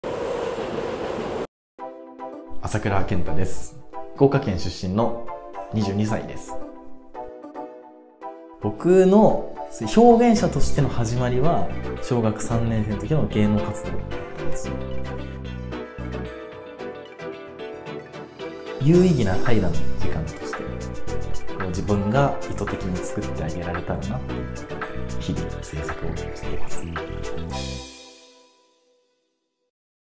アーティストの魅力をプロクリエイターが映像で表現【朝倉健太 ASAKURA Kenta】
V.aL 【バエル】
Visual + aL = V.aL
「作家×作品×クリエイティブ」の組み合わせからなる新しいアートメディア。
アーティスト自身の魅力をプロのクリエイターが映像や写真で表現する。
これまで語られなかった想いや作家人生を深ぼるインタビューをYouTubeにて配信中。

V.aL第一弾は「朝倉健太 ASAKURA Kenta」とaL Baseで10月20日〜開催される個展『 Need to Rest 』のコラボレーション

Vol.01
朝倉健太 ASAKURA Kenta

▼ Profile
福岡県出身、22歳。
役者として劇団四季やドラマ作品に出演を経て、「自己表現」の追求を目指し、2021年から本格的にアーティスト活動を開始する。